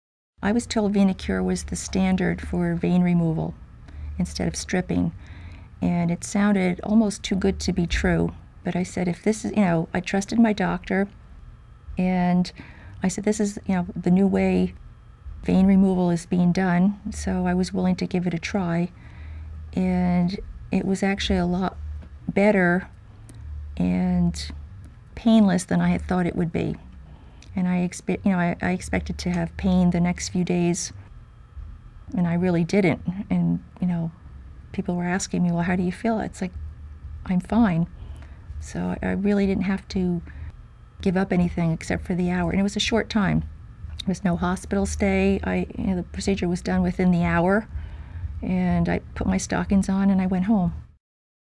Patient Explains Procedure and After.wmv
A patient explains how easy and pain-free varicose vein treatment can be and describes her experience with the VenaCure EVLT procedure and recovery.